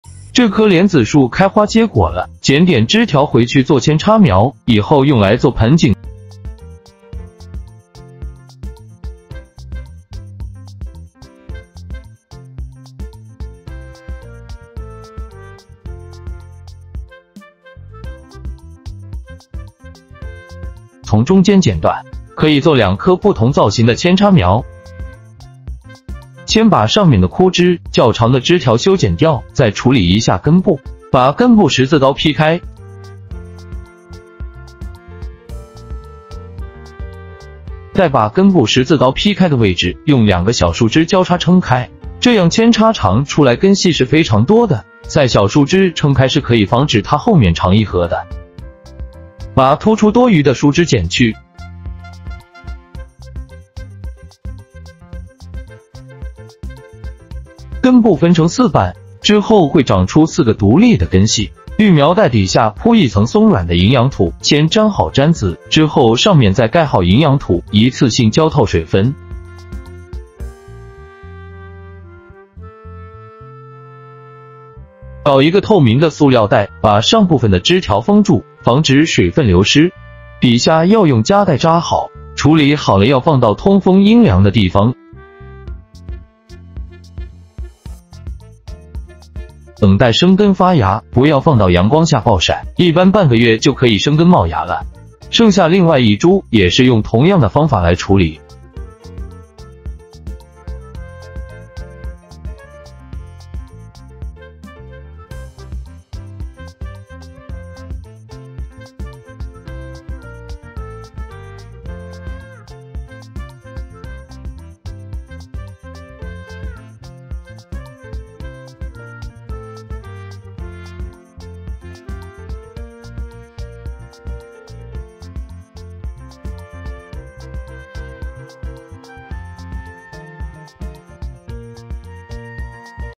扡插生根最快方法，15天爆根多成活率高Cutting is the fastest way to take root. The survival rate is high after 15 days，
Cutting is the fastest way to take root. The root is cut twice and the survival rate is high after 15 days，扡插生根最快方法，根部切2刀，15天爆根多成活率高